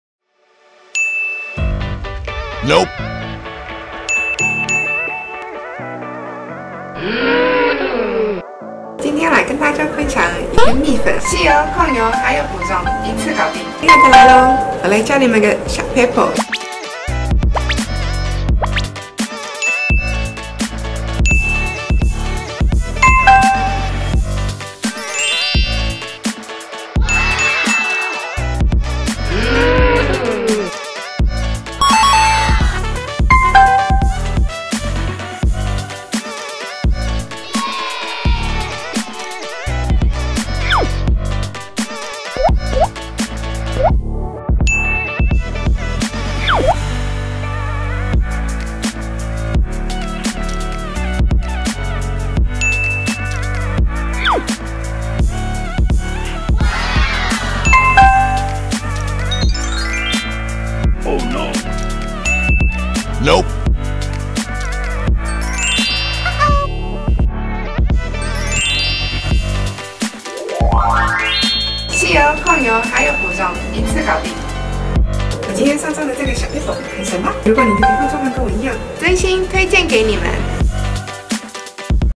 【MKUP美咖】油肌一生推蜜粉💗＄２６６🆙／入
「一拍０毛孔０油光」超強持效力不易脫妝
誰說油肌就要承受出油爆汗油膩膩底妝🤦
超持效蜜粉解救 控油輕薄不再泛油光✨
提拉式拍在臉頰再均勻刷開 填補瑕疵立體輪廓
首創 皮脂吸附粉末 抗汗不怕水💧
控油、吸油、補妝 一顆蜜粉就搞定❤️
廣受油肌女孩大推 用過就回不去了～
💟一拍柔焦毛孔
💟粉質細緻，清爽薄透
💟親膚配方，溫和不易刺激
MKUP分享王社團不可錯過！
※因為每個人膚色、膚況不同，產品效果使用起來會不一樣唷！